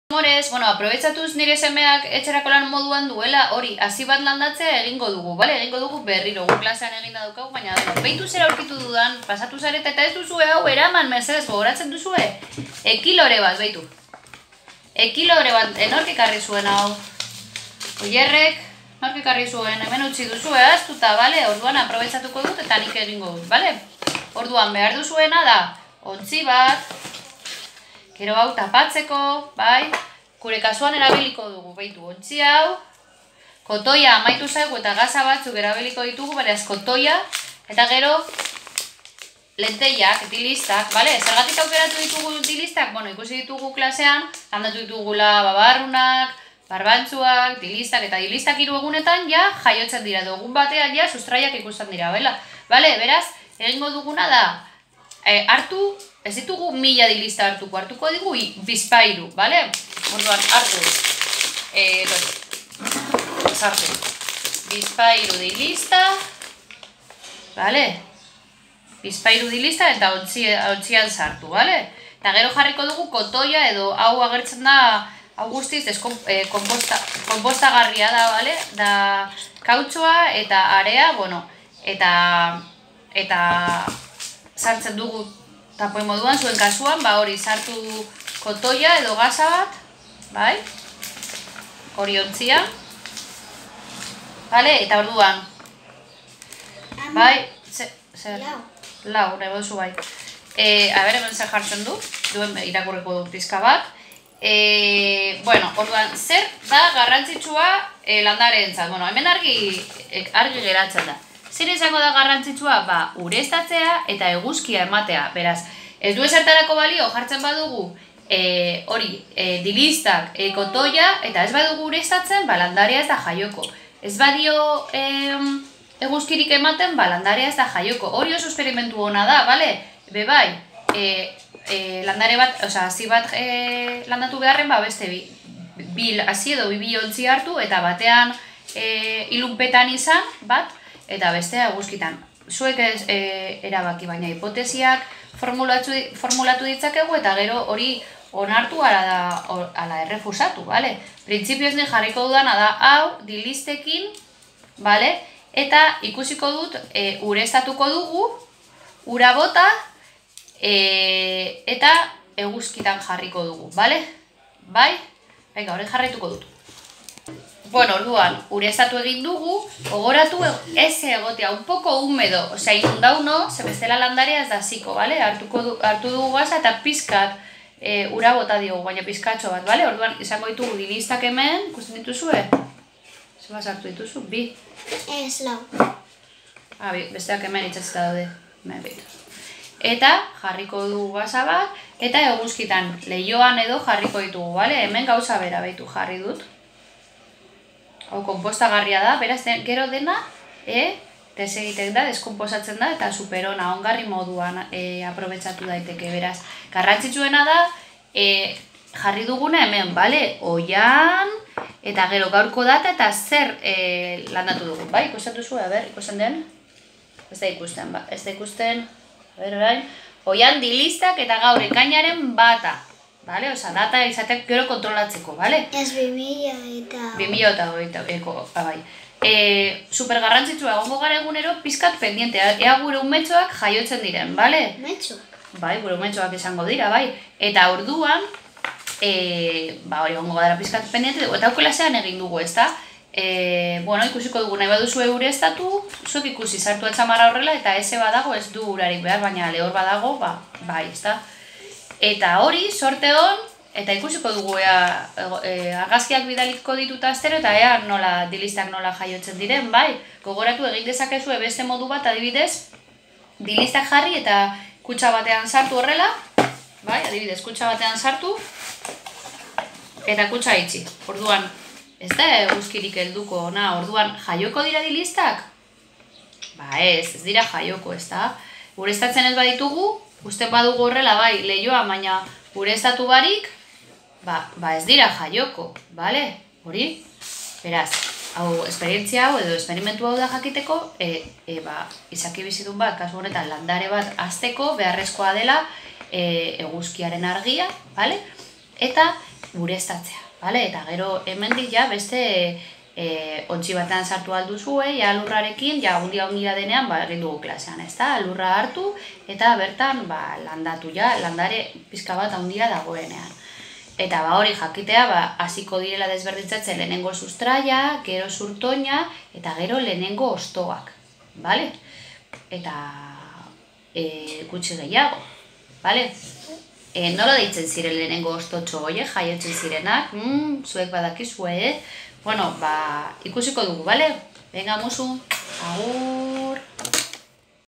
Landare baten atalak. Partes de una planta🌱
Dilistak landatuko ditugu. Plantaremos lentejas.
Primero saldrán las raíces luego observaremos el tallo y luego los cotiledones que se convertirán en hojas. Lehenengo sustraiak aterako dira, gero zurtoina eta gero kotiledoiak hostoak izango dira.